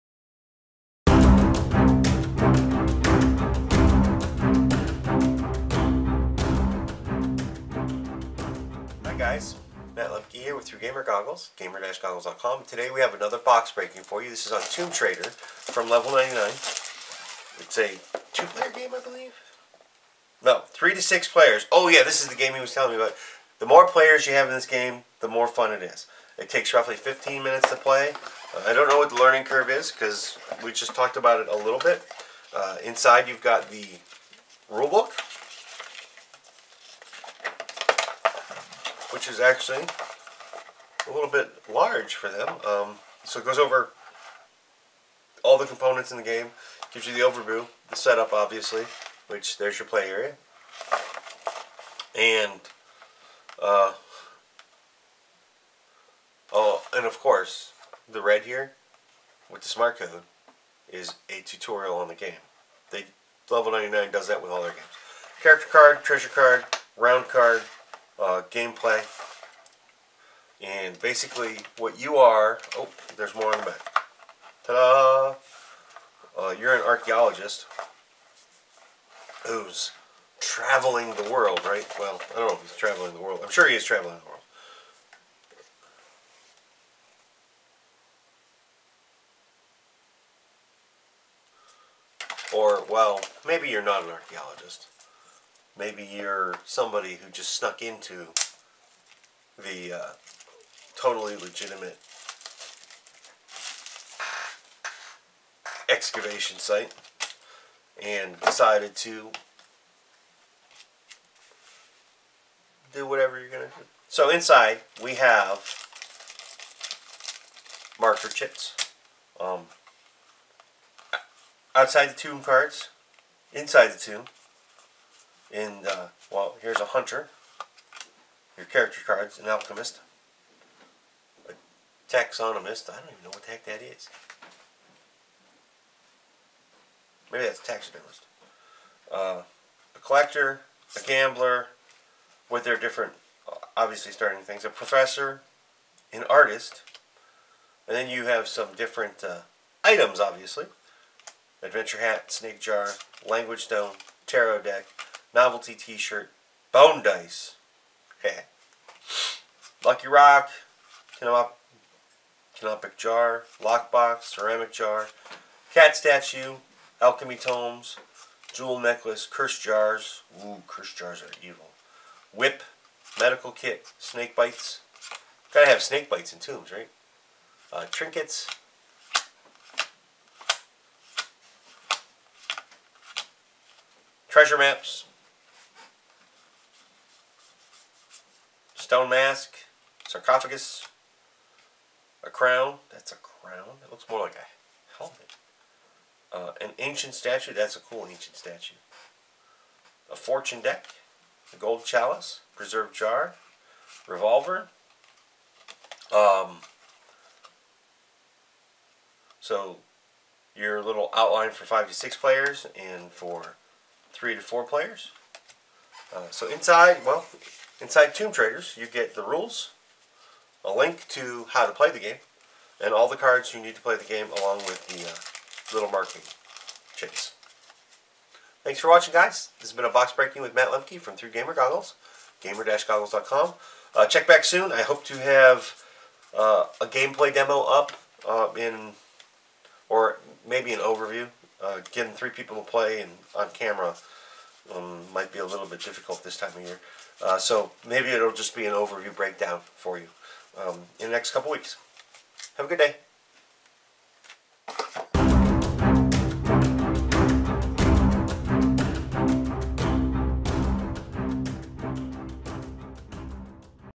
Box Breaking 222: Tomb Trader from Level 99
In this Box Breaking Matt Takes a look at Tomb Trader. The game that puts players in an archaeological dig with th ehopes of getting rich.